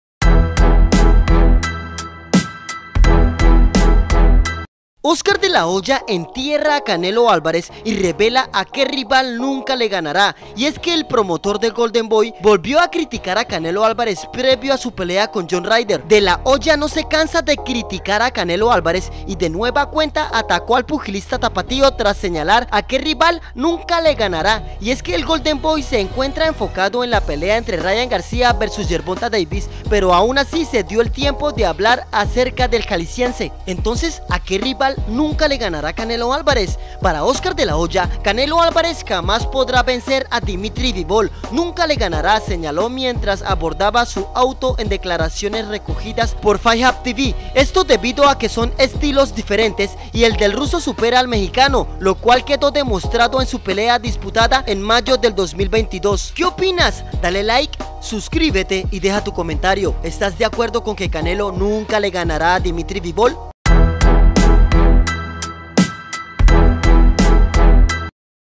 ¡HUMILLACIÓN! OSCAR DE LA HOYA ENTIERRA A CANELO ÁLVAREZ Y REVELA A QUÉ RIVAL NUNCA LE GANARÁ
OSCAR DE LA HOYA ENTIERRA A CANELO ÁLVAREZ Y REVELA A QUÉ RIVAL NUNCA LE GANARÁ